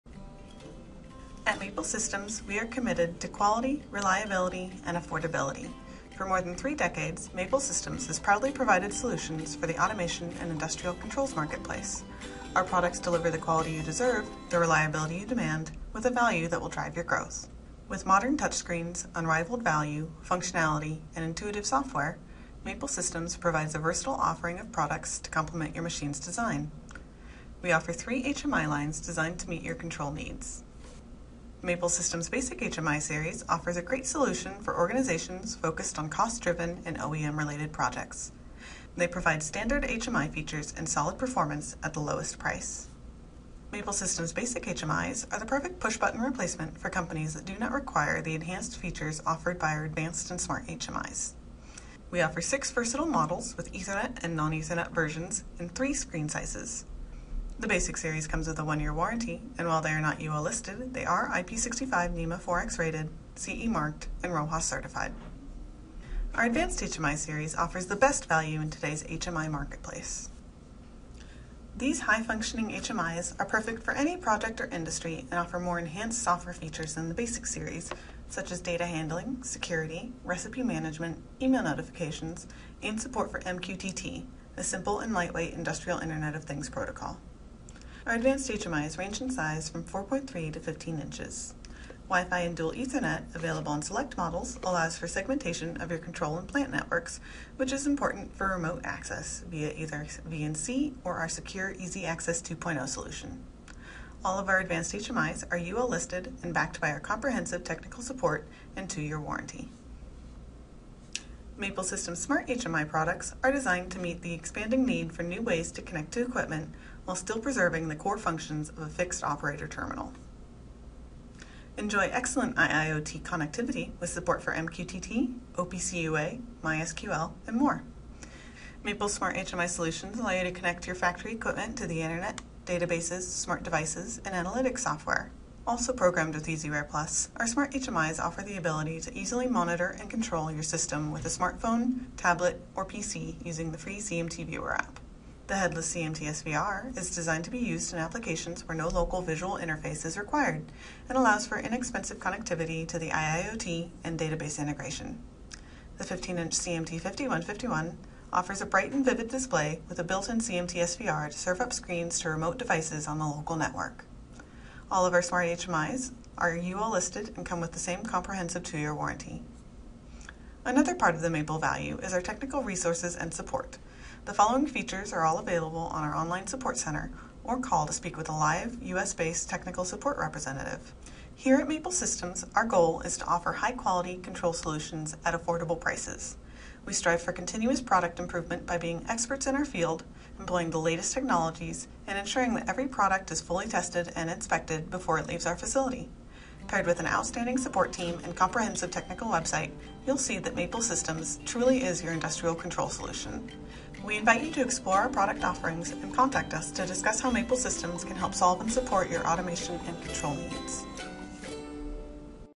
Maple Systems HMI Overview
See an overview of our Basic, Advanced, and Smart HMIs.